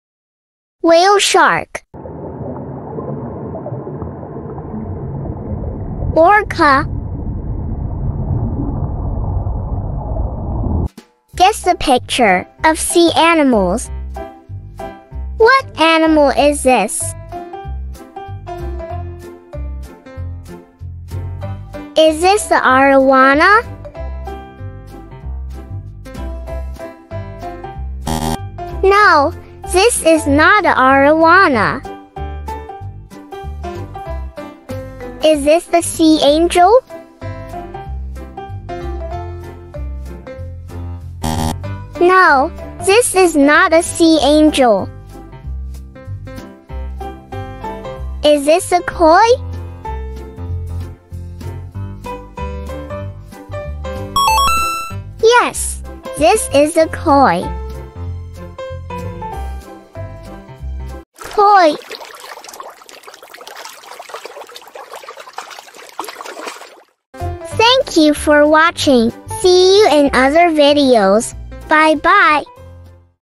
Guess The Whale Shark, Orca, Koi, Arowana, Sea Angel - Sea Animals
This time we have some sea animals, namely whale shark, orca, koi, arowana & sea angel.